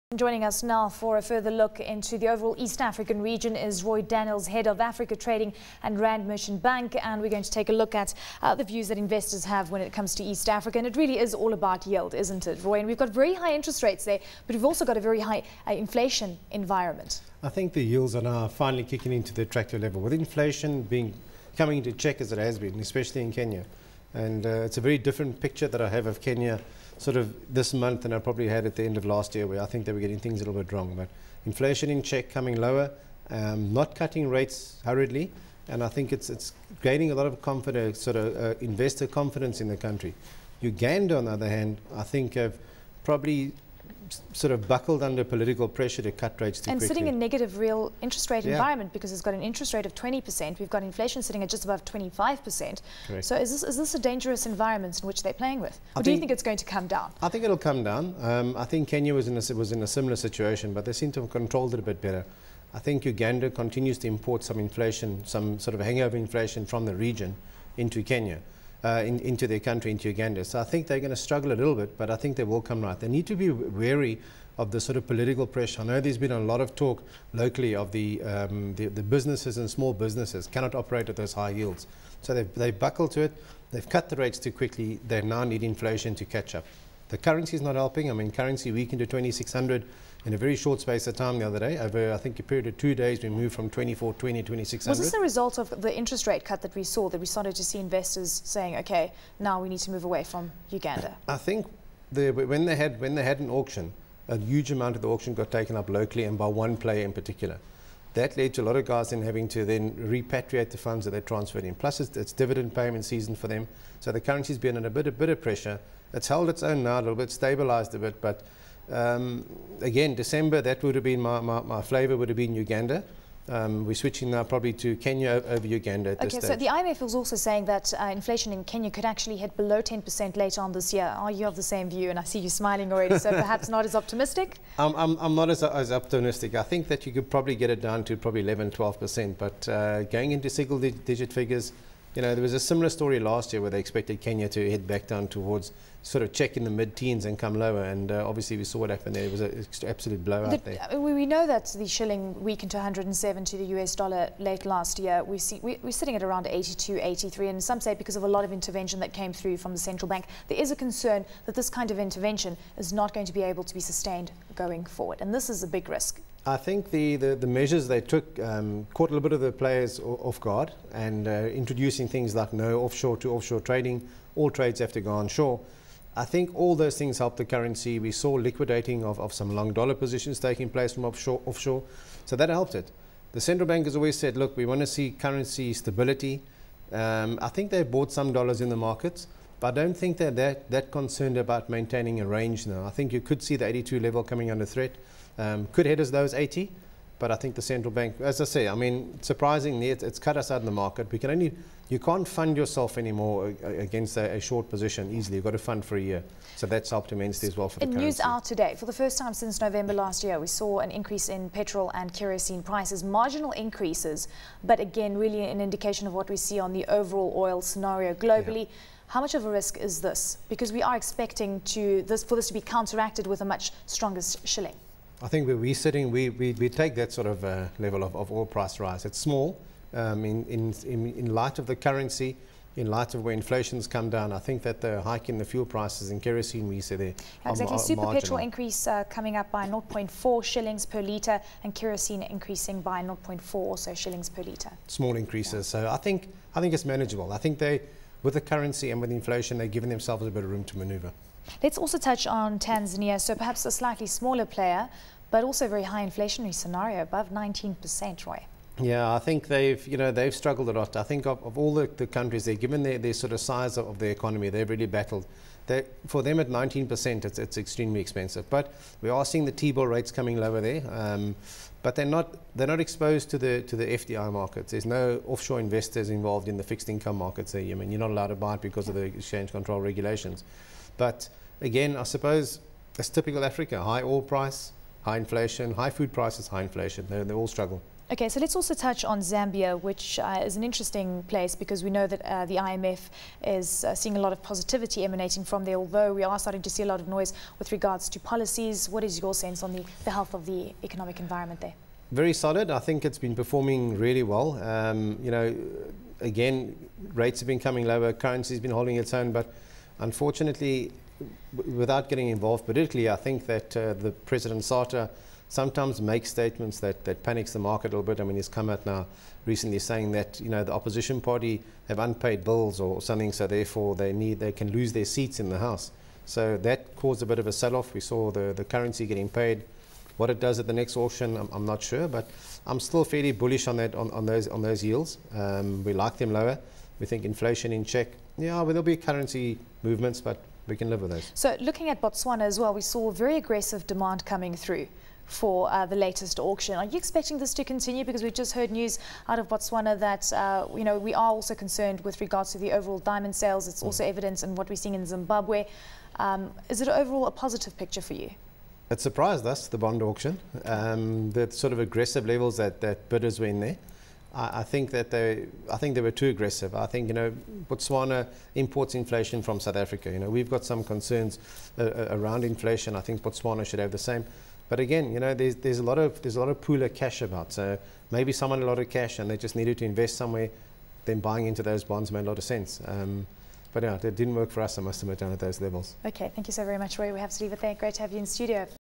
Investor Outlook for East Africa with Roy Daniels
ABN's Eleni Giokos is joined in studio by Roy Daniels, Head of Africa Trading at Rand Merchant Bank, to take a look at investor outlook for eastern Africa.